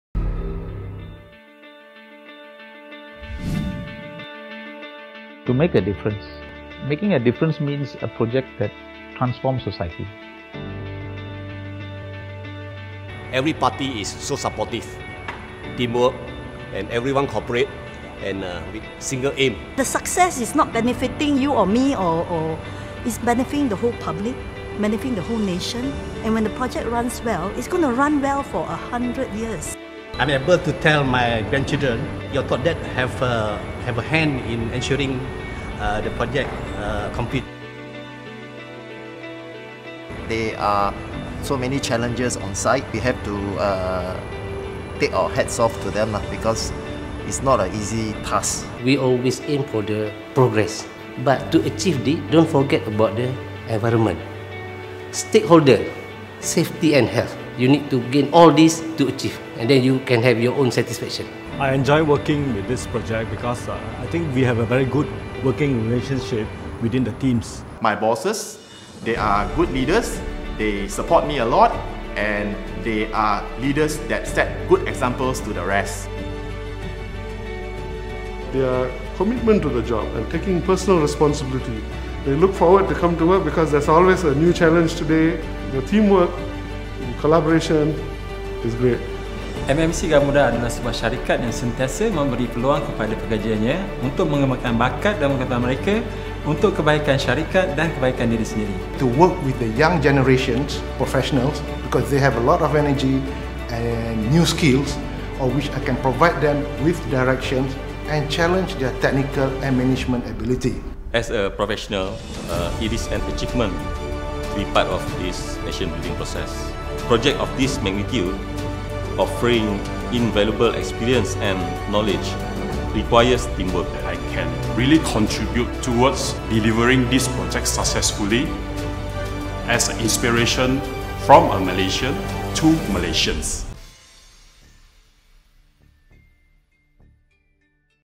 Working on the MRT SSP Line
With the MRT SSP Line all geared for the next challenging phase, we asked the people behind the project what it means for them to be a part of a nation building project like the KVMRT.